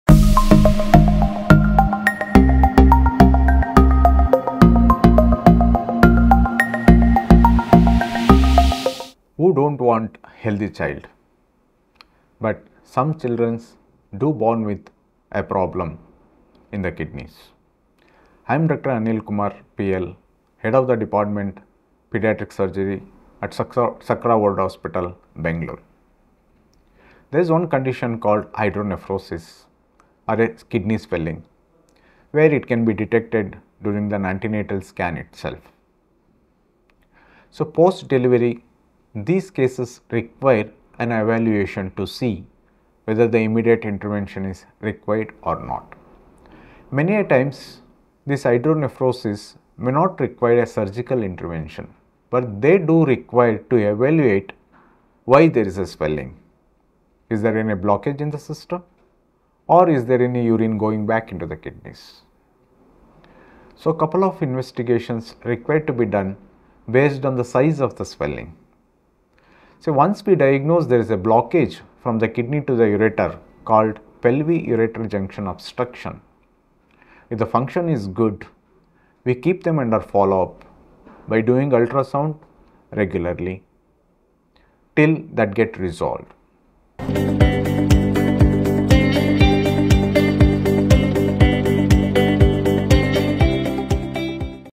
Dr. Anil Kumar on Hydronephrosis- Diagnosis | Sakra World Hospital
Exploring paediatric kidney concerns, Dr. Anil Kumar, Senior Consultant & Head of Paediatric Surgery at Sakra World Hospital, Banglore speaks about hydronephrosis detected in antenatal scans.

Post-delivery, cases undergo thorough evaluation for potential interventions, as he examines its causes.